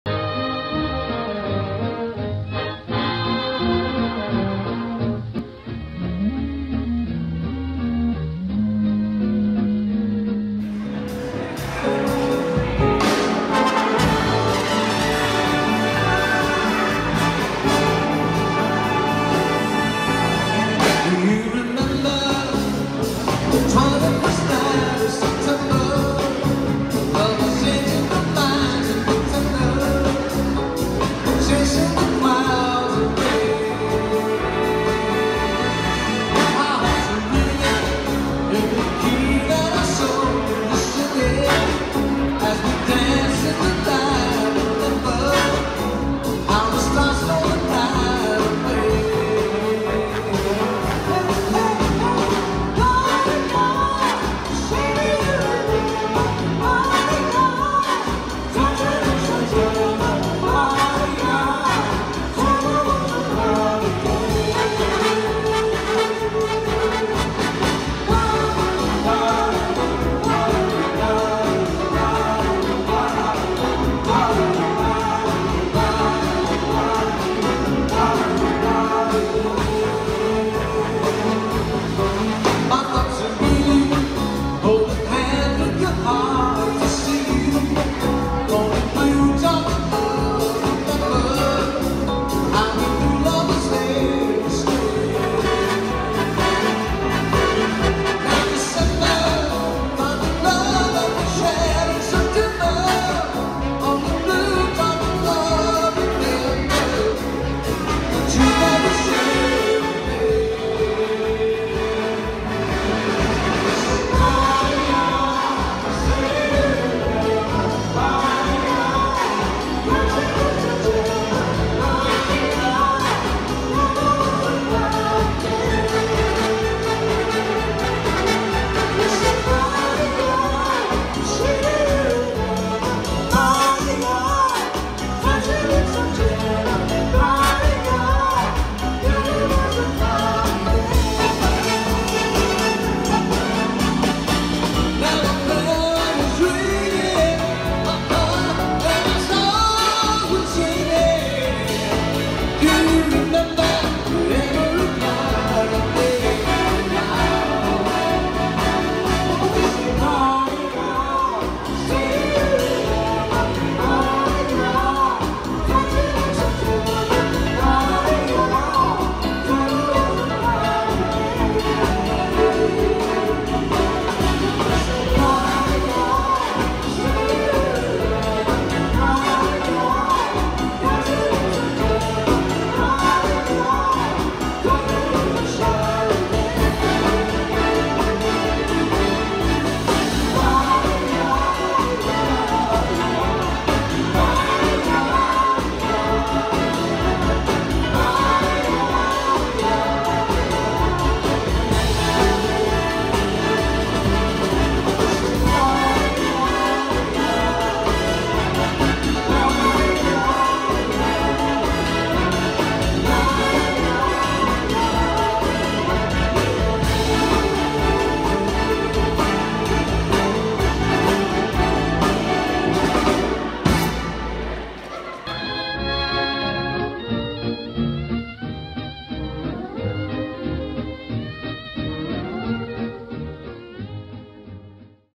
The Joey Thomas Big Band return to the Diamond Dance - April 2017
The Joey Thomas Big Band play the April 2017 Diamond Dance.